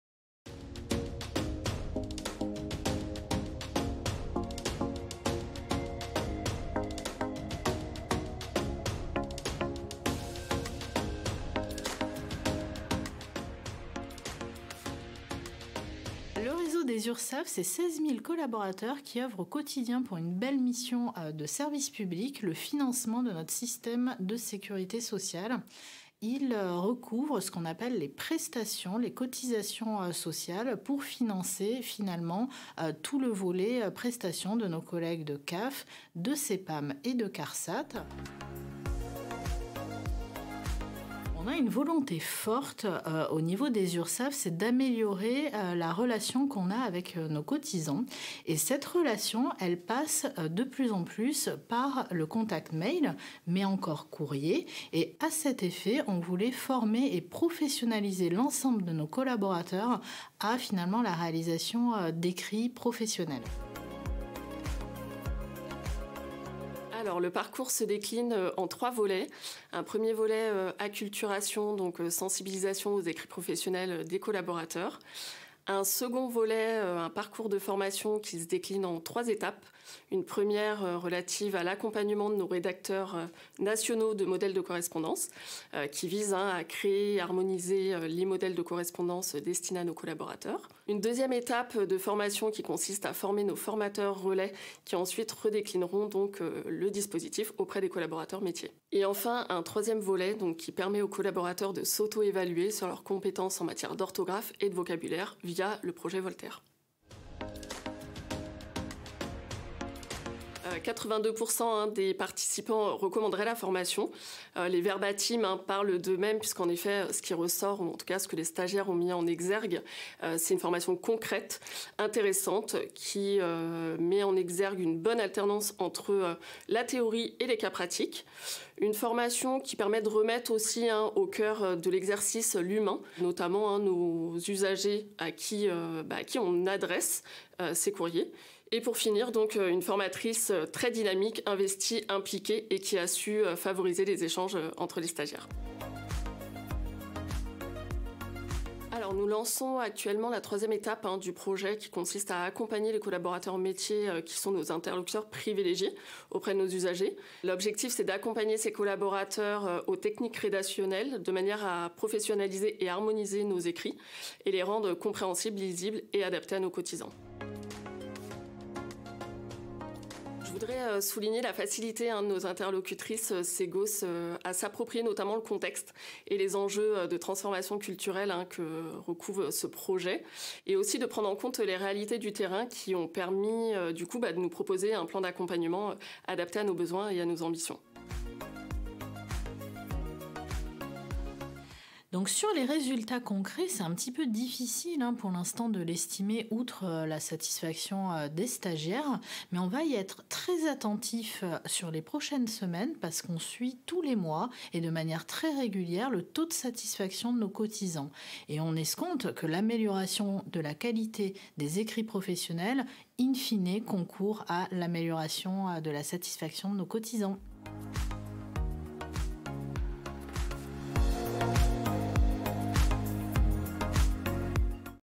Témoignage URSSAF : Améliorer les écrits adressés aux usagers
Le réseau des Urssaf s’est fixé une ambition forte d’amélioration de la qualité de service aux cotisants. Un des enjeux clé consiste à améliorer l’ensemble des courriers et courriels qui leur sont adressés. Les équipes Cegos ont mis en place un parcours de formation pour harmoniser et professionnaliser la qualité des écrits des collaborateurs. Découvrez le témoignage d'Anna Baldecchi,  responsable RH réseau, et de Valentine Maisonnave, Responsable du Pôle Développement RH Réseau, URSSAF - Caisse nationale.